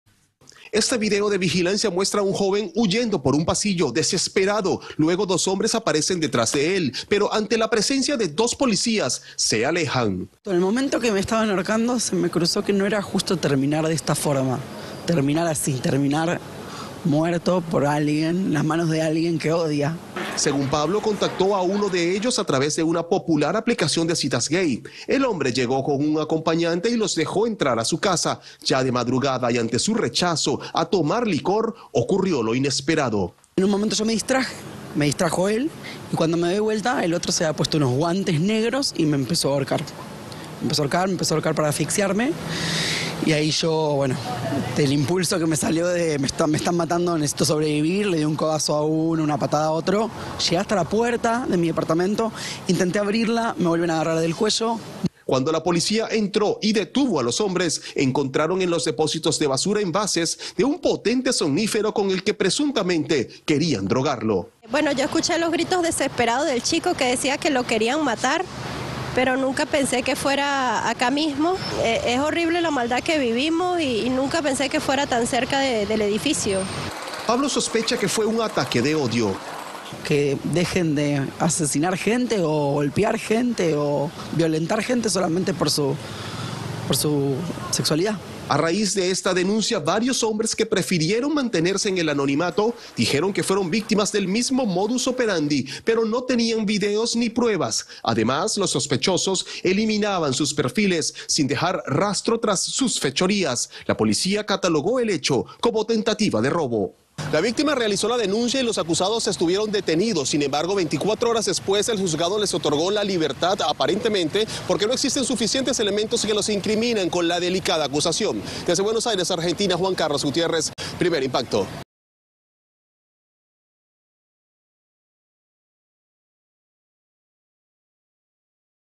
"Se puso guantes negros y comenzó a ahorcarme": consiguió una cita por una app y casi muere
Pablo D'Elía cuenta que invitó a su casa a un hombre por medio de una app de citas. Asegura que cuando lo recibió estaba acompañado y que estando en el apartamento de él intentaron asesinarlo.

Las noticias y reportajes más impactantes que ocurren en Estados Unidos y el mundo, presentadas por Michelle Galván y Pamela Silva.